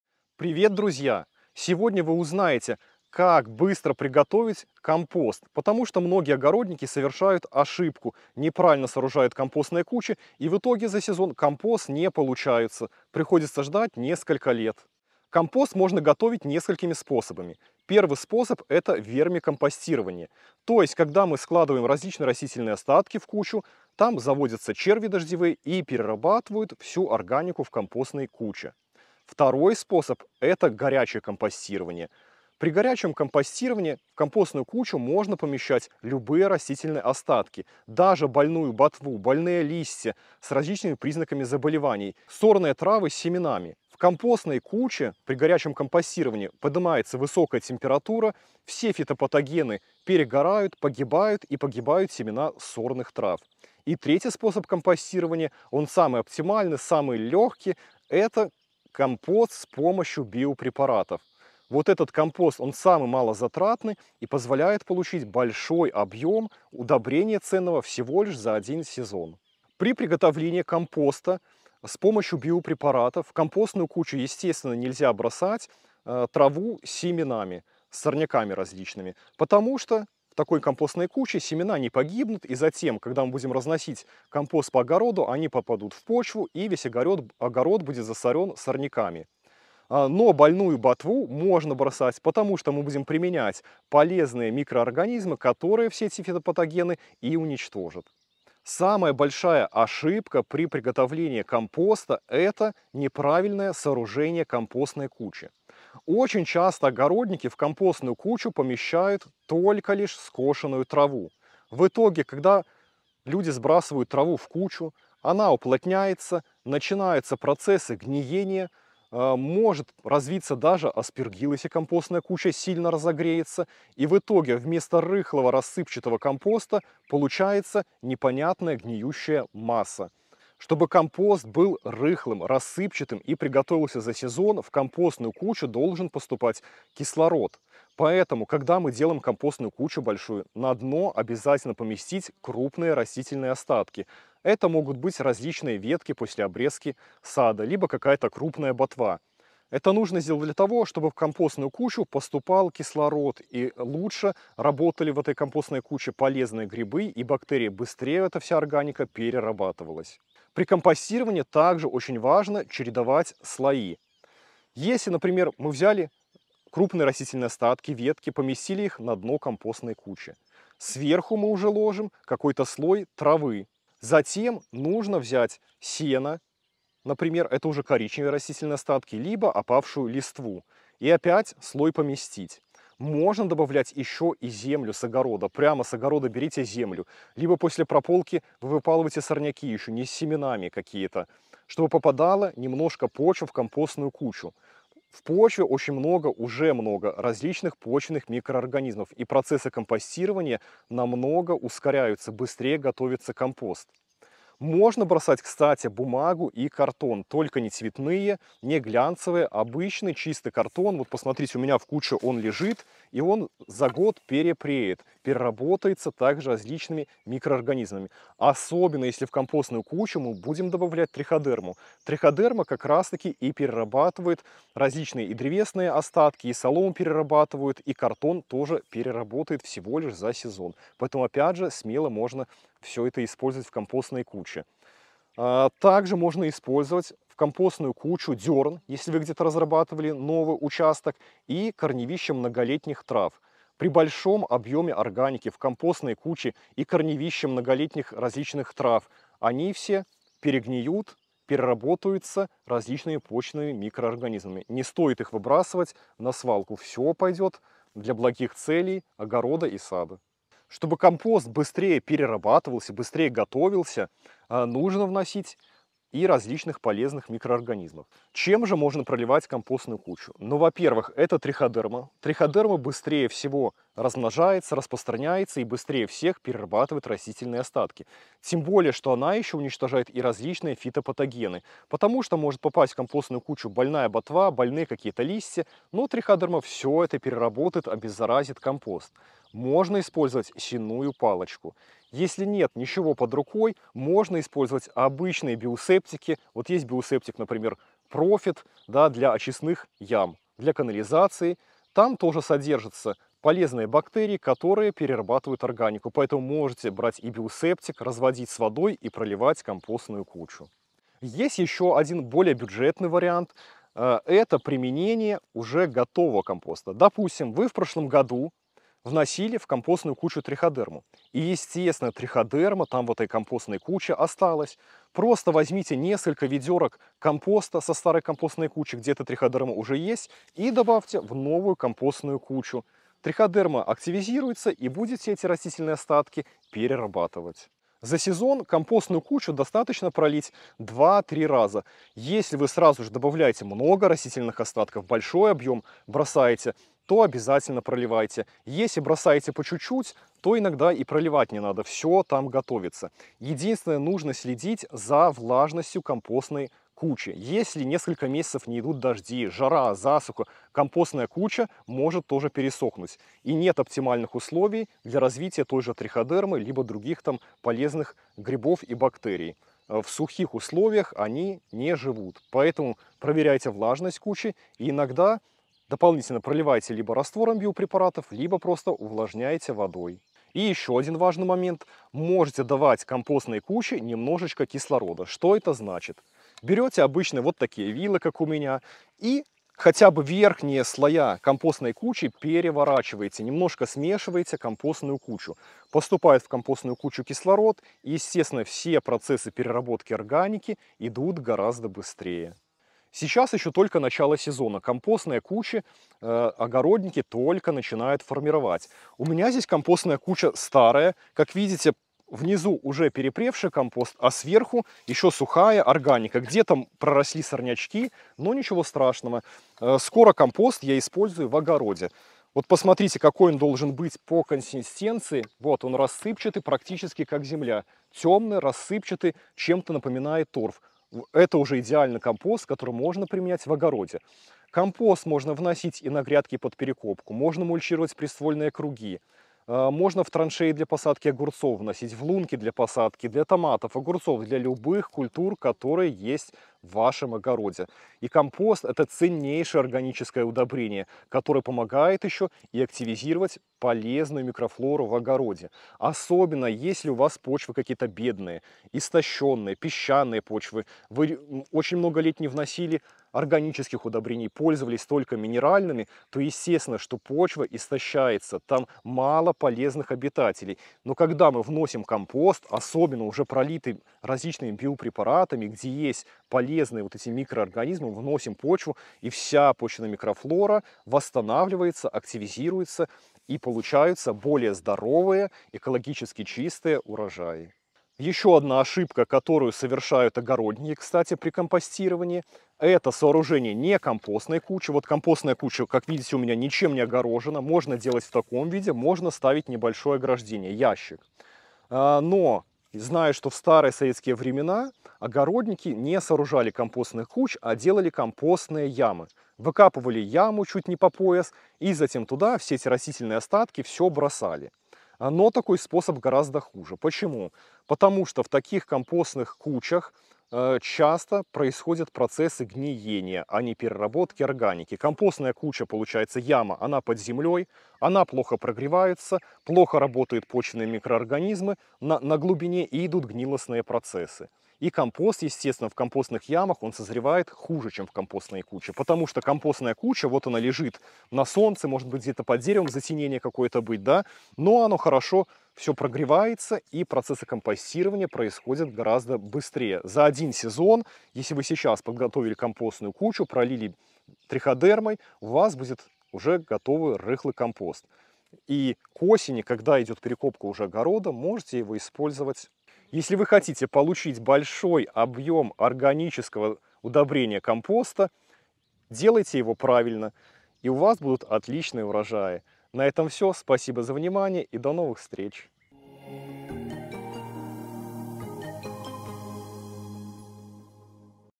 САМАЯ БОЛЬШАЯ ОШИБКА ПРИ КОМПОСТИРОВАНИИ, из-за которой компост не зреет и несет опасность здоровью
Как быстро и правильно приготовить компост? Многие дачники совершают ошибки при изготовлении компостной кучи, и компост не успеет созреть за один сезон, а еще в таком компосте могут завестись вредные микроорганизмы. В этом видео опытный садовод Дмитрий Кастрицкий рассказал об основных ошибках при приготовления компоста и как их избежать.

00:00 В этом видео
00:18 Вермикомпостирование
00:32 Горячее компостирование
00:56 Компостирование с биопрепаратами
01:41 Самая большая ошибка при приготовлении компоста
02:20 Что обязательно надо положить на дно компостной кучи
02:46 Чередуйте слои при складировании компоста
04:22 Чем пролить компост для его обеззараживания
05:18 Бюджетный способ ускорения компостирования
05:55 Следите за влажностью компостной кучи
06:24 Обеспечьте компостную кучу кислородом
07:15 Как применять компост в саду и огороде
08:15 О компостных ямах